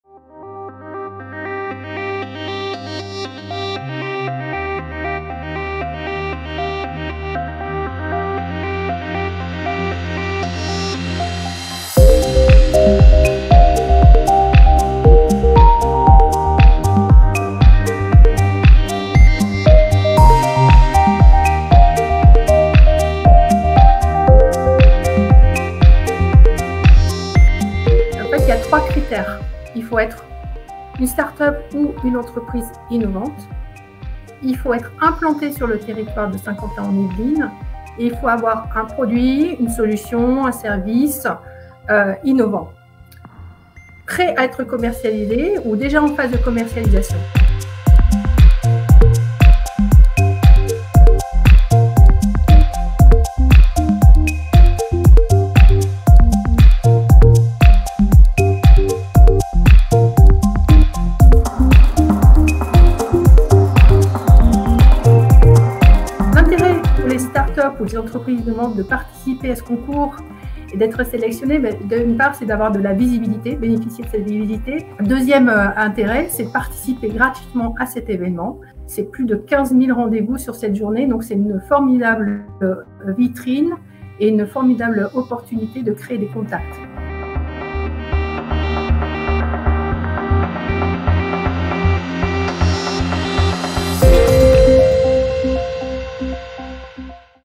Yvelines | Un concours pour participer au Techinnov 2022 !
C’est une tradition pour les acteurs de l’innovation. Le rendez-vous Techinnov les rassemble chaque année. Cette journée de business et de rencontres aura lieu le 23 mars 2022 au Parc Floral de Paris. Dans ce contexte, Saint-Quentin-en-Yvelines organise un concours pour faire gagner des places à des start-up pour y participer. Les détails dans cette séquence décryptage.